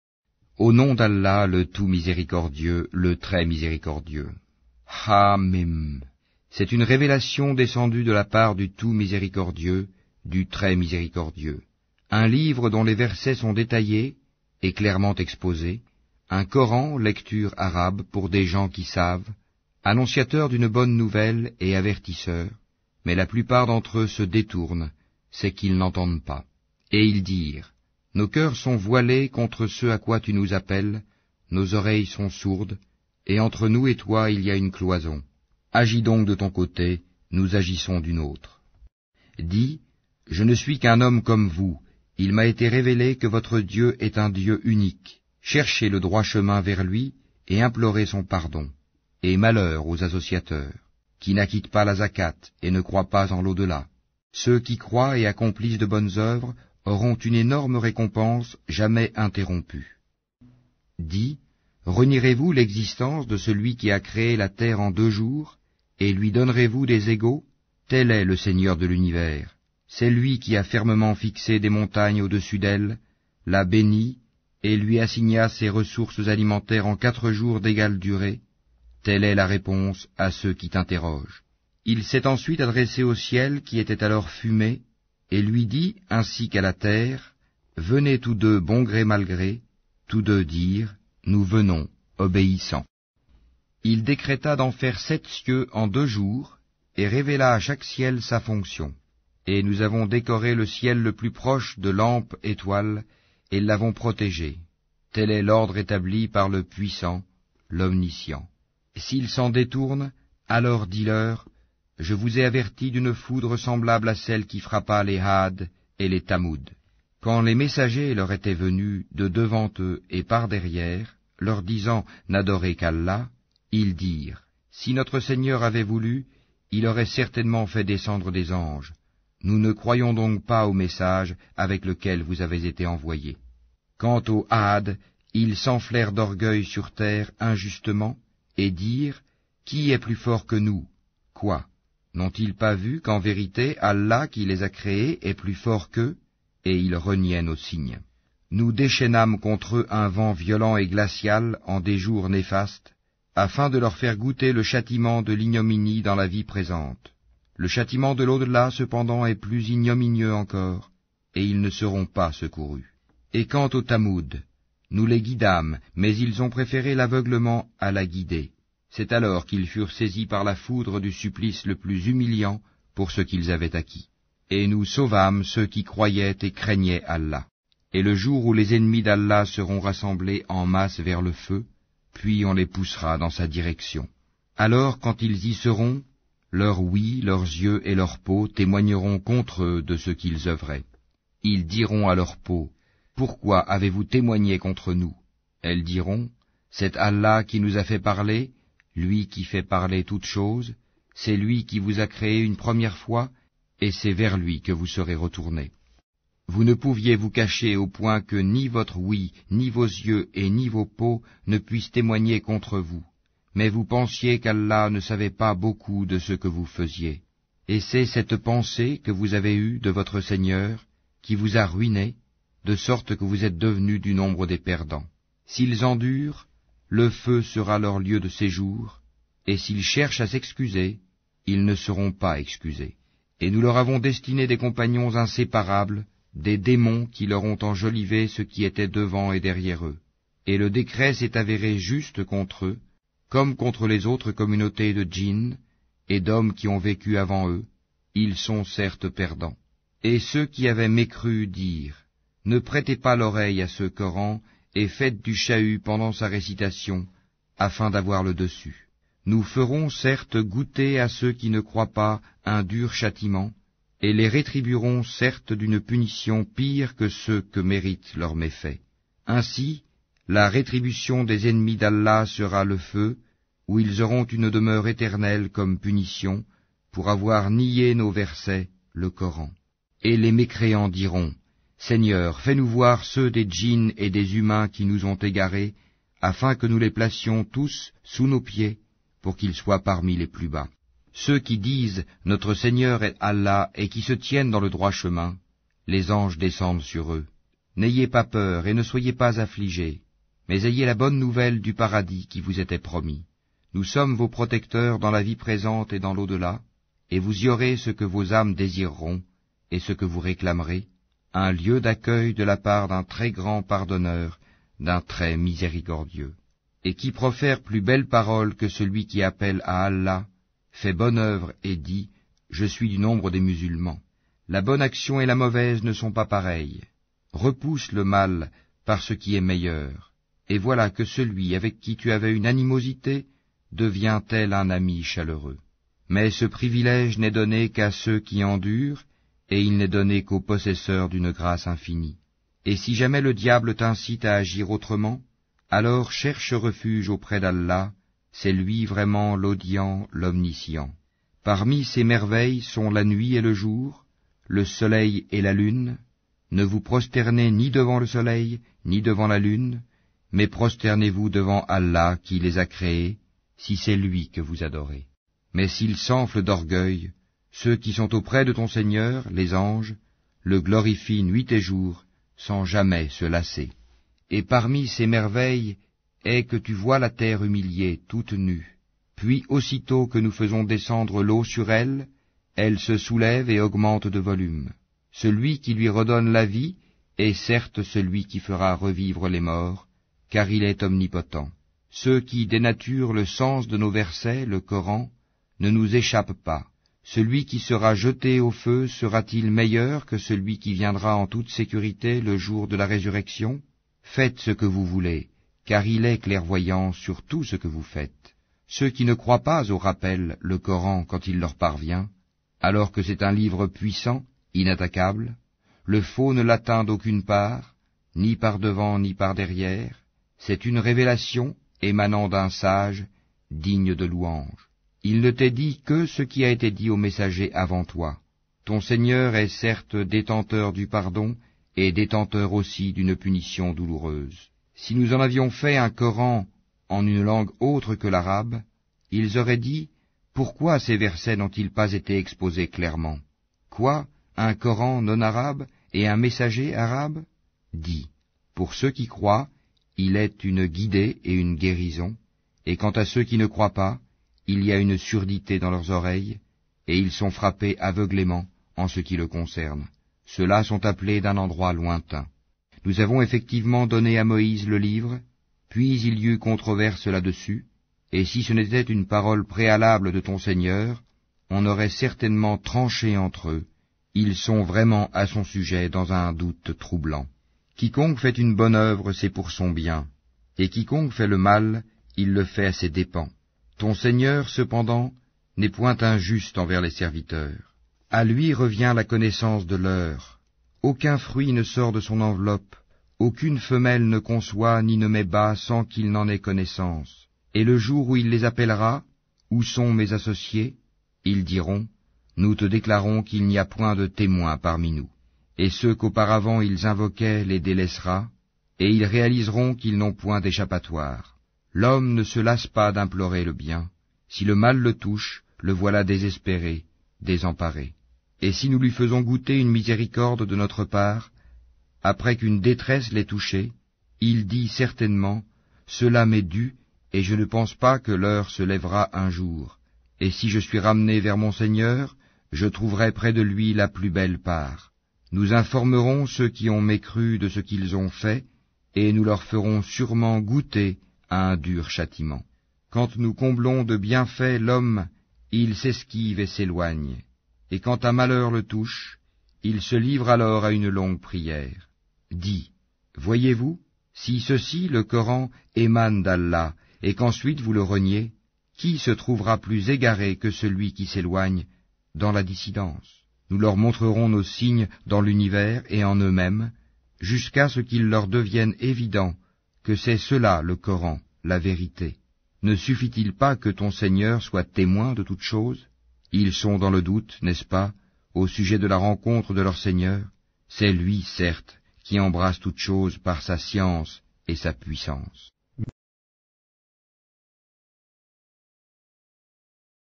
CORAN FRANÇAIS - SOURATE 41 - LES VERSETS DÉTAILLÉS (récitation 🇫🇷 + lecture)
1. Ḥ’ā, Mīm(2).

2. [C’est] une Révélation descendue de la part du Tout Miséricordieux, du Très Miséricordieux.

3. Un Livre dont les versets sont détaillés (et clairement exposés), un Coran [lecture] arabe pour des gens qui savent,

4. annonciateur [d’une bonne nouvelle] et avertisseur. Mais la plupart d’entre eux se détournent; c’est qu’ils n’entendent pas.

5. Et ils dirent: «Nos cœurs sont voilés contre ce à quoi tu nous appelles, nos oreilles sont sourdes. Et entre nous et toi, il y a une cloison. Agis donc de ton côté; nous agissons du nôtre.»

6. Dis: «Je ne suis qu’un homme comme vous. Il m’a été révélé que votre Dieu est un Dieu unique. Cherchez le droit chemin vers Lui et implorez son pardon». Et malheur aux Associateurs

7. qui n’acquittent pas la Zakāt et ne croient pas en l’au-delà!

8. Ceux qui croient et accomplissent de bonnes œuvres auront une énorme récompense jamais interrompue.

9. Dis: «Renierez-vous [l’existence] de celui qui a créé la terre en deux jours et Lui donnerez-vous des égaux? Tel est le Seigneur de l’univers,

10. c’est Lui qui a fermement fixé des montagnes au-dessus d’elle, l’a bénie et lui assigna ses ressources alimentaires en quatre jours d’égale durée. [Telle est la réponse] à ceux qui t’interrogent.

11. Il S’est ensuite adressé au ciel qui était alors fumée et lui dit, ainsi qu’à la terre: «Venez tous deux, bon gré, mal gré». Tous deux dirent: «Nous venons obéissants».

12. Il décréta d’en faire sept cieux en deux jours et révéla à chaque ciel sa fonction(3). Et Nous avons décoré le ciel le plus proche de lampes [étoiles] et l’avons protégé. Tel est l’Ordre établi par le Puissant, l’Omniscient.

13. S’ils s’en détournent, alors dis-leur; «Je vous ai avertis d’une foudre semblable à celle qui frappa les ˒Aad et les Ṯamūd».

14. Quand les Messagers leur étaient venus, de devant eux et par derrière(4), [leur disant]: «N’adorez qu’Allah», ils dirent: «Si notre Seigneur avait voulu, Il aurait certainement fait descendre des Anges. Nous ne croyons donc pas [au message] avec lequel vous avez été envoyés».

15. Quant aux ˒Aad, ils s’enflèrent d’orgueil sur terre injustement et dirent: «Qui est plus fort que nous?» Quoi! N’ont-ils pas vu qu’en vérité Allah qui les a créés est plus fort qu’eux? Et ils reniaient Nos signes.

16. Nous déchaînâmes contre eux un vent violent et glacial en des jours néfastes, afin de leur faire goûter le châtiment de l’ignominie dans la vie présente. Le châtiment de l’au-delà cependant est plus ignominieux encore, et ils ne seront pas secourus.

17. Et quant aux Ṯamūd, Nous les guidâmes; mais ils ont préféré l’aveuglement à la guidée. C’est alors qu’ils furent saisis par la foudre du supplice le plus humiliant pour ce qu’ils avaient acquis.

18. Et Nous sauvâmes ceux qui croyaient et craignaient Allah.

19. Et le jour où les ennemis d’Allah seront rassemblés en masse vers le Feu... Puis on les poussera [dans sa direction].

20. Alors, quand ils y seront, leur ouïe, leurs yeux et leurs peaux témoigneront contre eux de ce qu’ils œuvraient.

21. Ils diront à leurs peaux: «Pourquoi avez-vous témoigné contre nous?» Elles diront: «C’est Allah qui nous a fait parler, Lui qui fait parler toute chose. C’est Lui qui vous a créés une première fois et c’est vers Lui que vous serez retournés».

22. Vous ne pouviez vous cacher au point que ni votre ouïe, ni vos yeux et ni vos peaux ne puissent témoigner contre vous. Mais vous pensiez qu’Allah ne savait pas beaucoup de ce que vous faisiez.

23. Et c’est cette pensée que vous avez eue de votre Seigneur, qui vous a ruinés, de sorte que vous êtes devenus du nombre des perdants.

24. S’ils endurent, le Feu sera leur lieu de séjour; et s’ils cherchent à s’excuser, ils ne seront pas excusés.

25. Et Nous leur avons destiné des compagnons inséparables [des démons] qui leur ont enjolivé ce qui était devant et derrière eux(5). Et le décret s’est avéré juste contre eux, comme contre les autres communautés de djinns et d’hommes qui ont vécu avant eux. Ils sont certes perdants!

26. Et ceux qui avaient mécru dirent: «Ne prêtez pas l’oreille à ce Coran, et faites du chahut (pendant sa récitation), afin d’avoir le dessus».

27. Nous ferons certes, goûter à ceux qui ne croient pas un dur châtiment, et les rétribuerons certes [d’une punition] pire que ce [que méritent] leurs méfaits.

28. Ainsi, la rétribution des ennemis d’Allah sera le Feu où ils auront une demeure éternelle, comme punition pour avoir nié Nos versets [le Coran].

29. Et les mécréants diront: «Seigneur, fais-nous voi